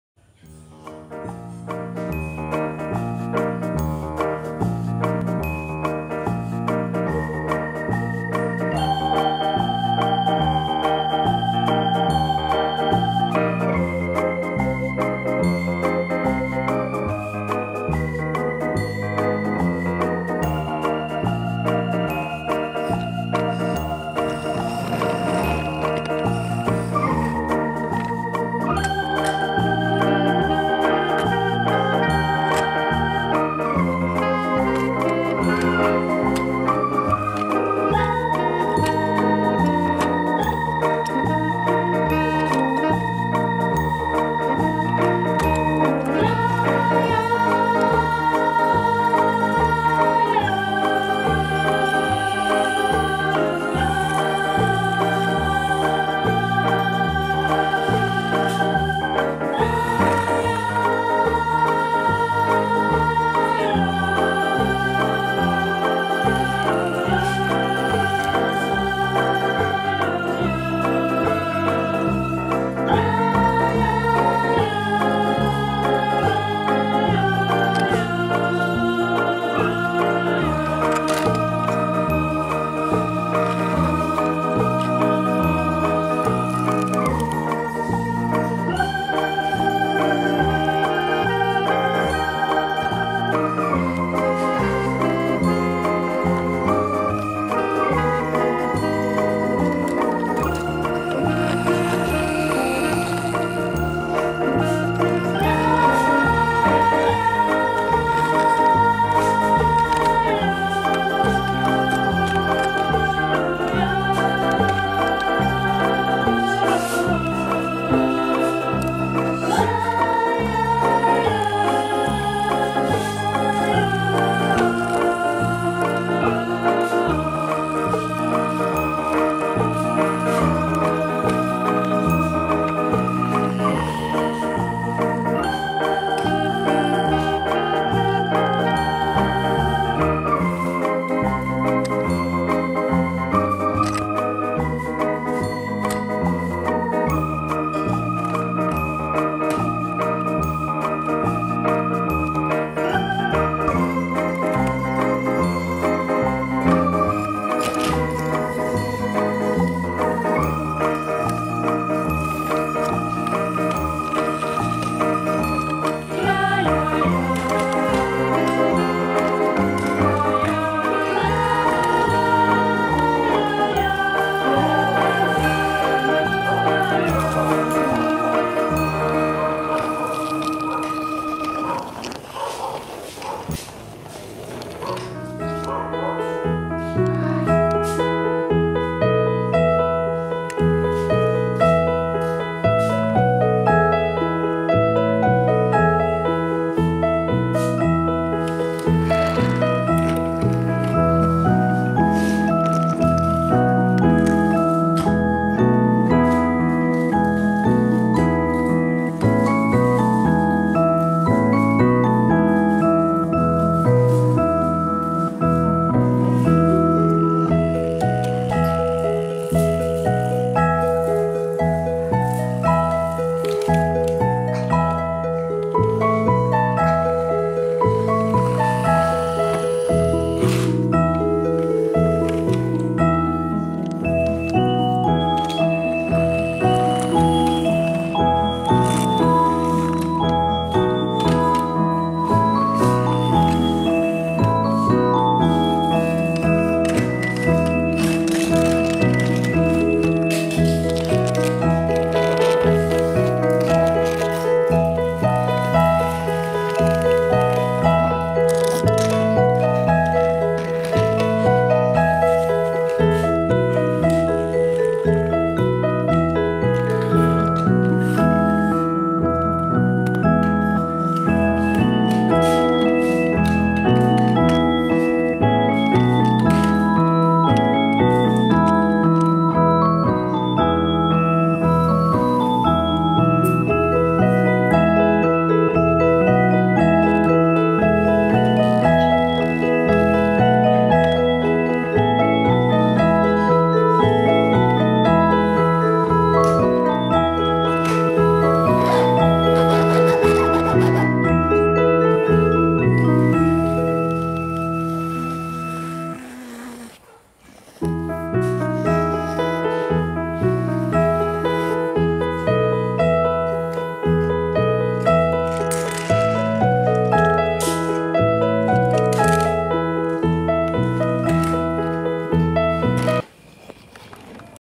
Baby Studio - planes transport trucks, cars | trucks toy
Baby Studio - planes transport trucks, cars | trucks toy | cars toy is an interesting video for kids, children. Today, we have some planes: small planes and a big plane. And there are some trucks: excavator trucks, tow trucks…there are some cars-small cars. They are cute kid toys. The planes transport trucks toy and cars toy. This is a hard work. And now, we watch together how these planes transport trucks and cars.